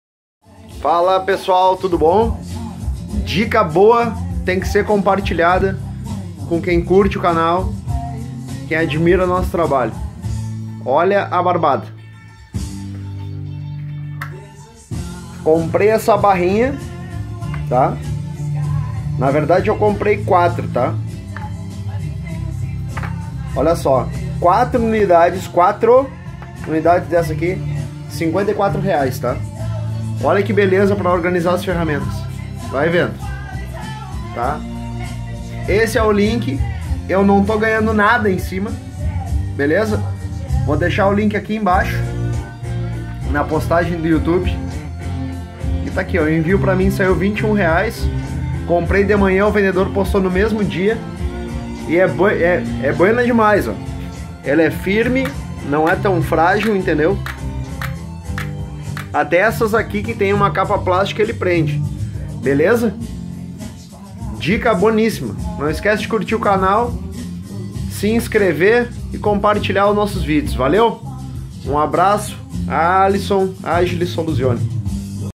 Organização de Ferramentas DICA baratíssima AgileSoluzioneResolvemos
"Restauramos os vintages que não tem prazo de validade"

Dica de como organizar suas ferramentas. LINK do anuncio, NAO tenho vinculo com o vendedor, porém ele foi muito rapido ao postar e o produto vale o valor.